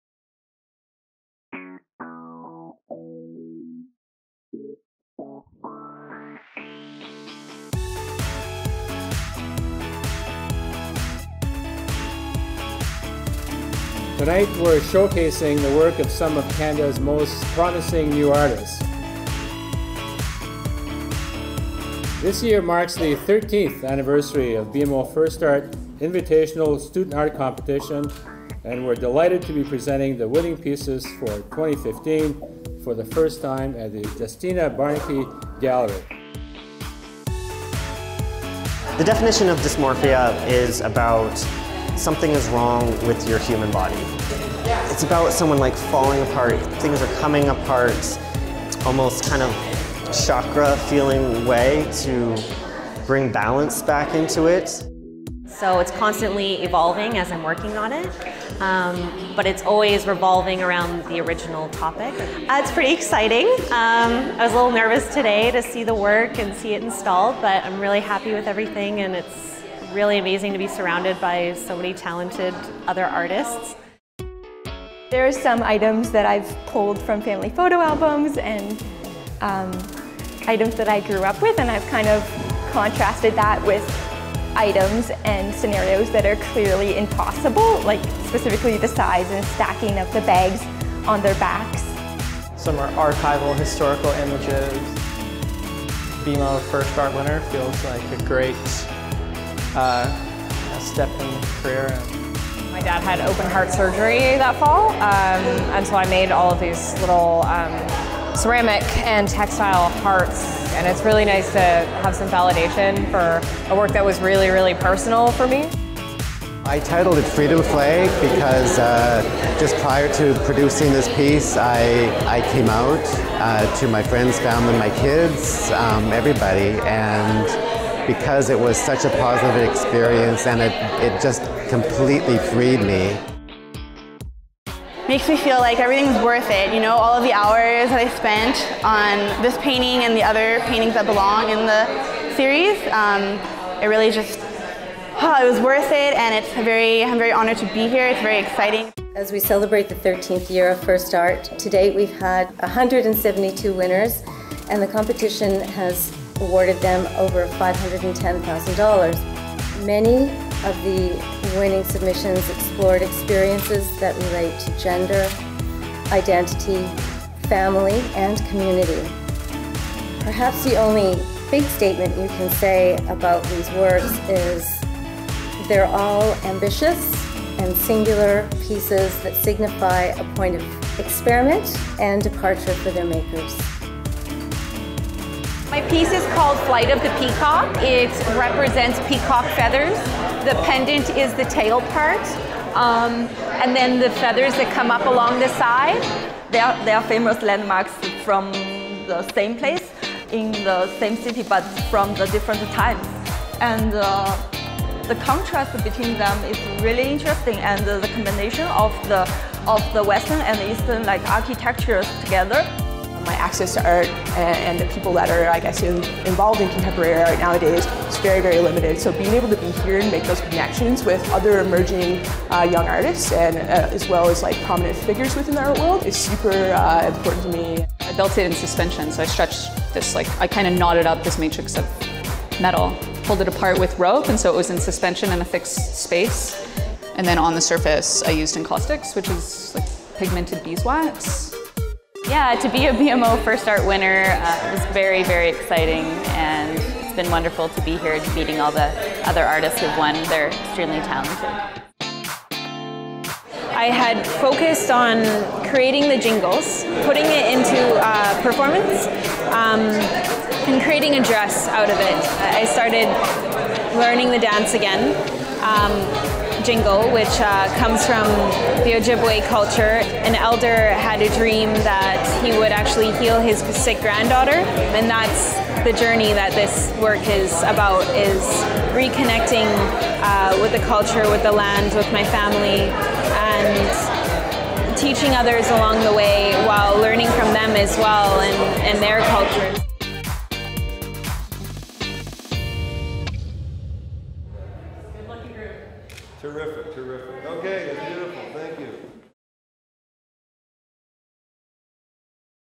1st Art! Invitational Student Art Competition | BMO | Bank of Montreal
BMO Financial Group's 1st Art! Invitational Student Art Competition celebrates the creativity of art students from post-secondary institutions across Canada. Deans and instructors of undergraduate certificate, diploma, and degree programs in visual art are invited to select from their graduating classes three students, whose ability and imagination place them 1st among their peers. A distinguished selection committee chooses a national winner and one winner from each eligible province and territory.

The national winner receives $10,000 and the regional winners each receive $5,000 for his or her artwork. The national winner’s work will become part of BMO’s corporate art collection, which includes historical and contemporary Canadian masters such as Kenojuak Ashevak, Emily Carr, Paterson Ewen, Tom Forestall, Marc-Aurèle Fortin, Lawren Harris, Thaddeus Holownia, David Milne, Margaret Priest, Denyse Thomasos, Ian Wallace and others. All winning entries will be on display at the 1st Art! 2015 exhibition, held at the Justina M. Barnicke Gallery from October to December 2015.